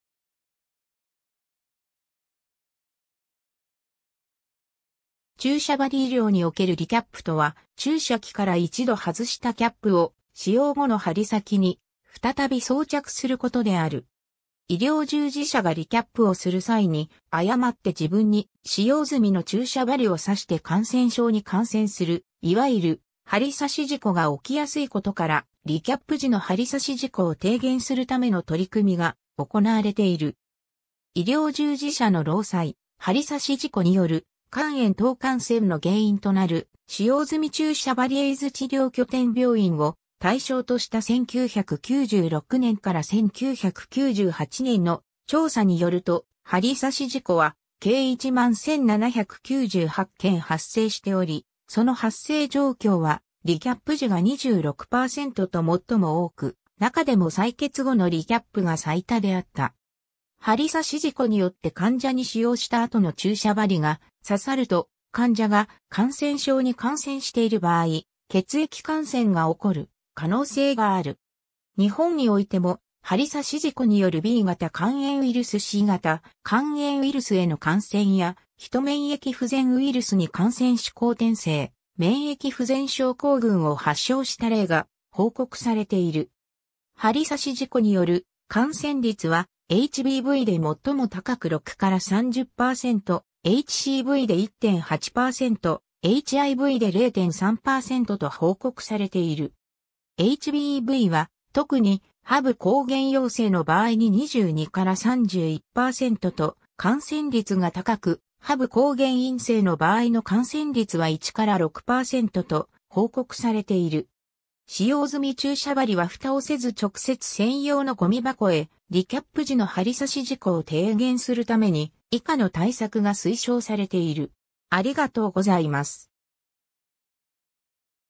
リキャップ
医療事故・医療事件
注射針 医療におけるリキャップ（英語: recap）とは、注射器から一度外したキャップ（蓋）を、使用後の針先に再び装着することである。
医療従事者がリキャップをする際に、誤って自分に使用済みの注射針を刺して感染症に感染する、いわゆる針刺し事故が起きやすいことから、リキャップ時の針刺し事故を低減するための取り組みが行われている。
 医療従事者の労災「針刺し事故」による肝炎等感染の原因となる使用済み注射針 エイズ治療拠点病院を対象とした1996年-1998年の調査によると、針刺し事故は計11,798件（病床100床あたり4件）発生しており、その発生状況はリキャップ時が26％と最も多く、中でも採血後のリキャップが最多であった。
 針刺し事故によって患者に使用した後の注射針が刺さると、患者が感染症に感染している場合、血液感染が起こる可能性がある。
日本においても、針刺し事故によるB型肝炎ウイルス（HBV）C型肝炎ウイルス（HCV）への感染や、ヒト免疫不全ウイルス（HIV）に感染し後天性免疫不全症候群（AIDS）を発症した例が報告されている（ただし、その原因がリキャップであるか否かは特定されていない）。
 針刺し事故による感染率はHBVで最も高く6〜30％、HCVで1.8％、HIVで0.3％と報告されている。
HBVは、特にHBe抗原陽性の場合に22〜31％と感染率が高く、HBe抗原陰性の場合の感染率は1〜6％と報告されている。
 使用済み注射針はフタをせず（リキャップせず）直接専用のゴミ箱へ リキャップ時の針刺し事故を低減するために、以下の対策が推奨されている。